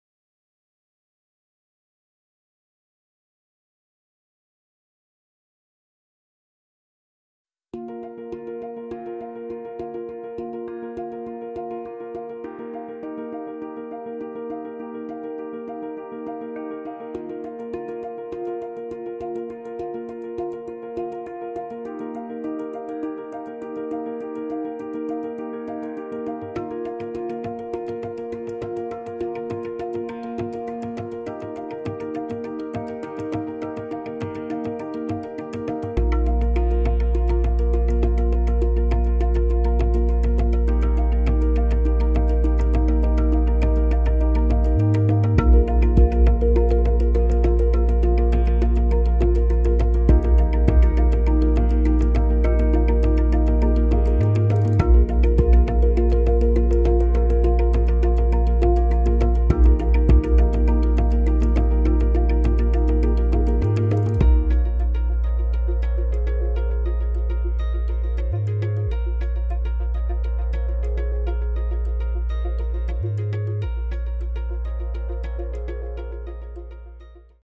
SPS IPC Drives Exhibition 2013 Stand Review
Mitsubishi Electric offers sushi, 3D video and a look at the next generation of its control and drive families at SPS IPC Drives Exhibition 2013.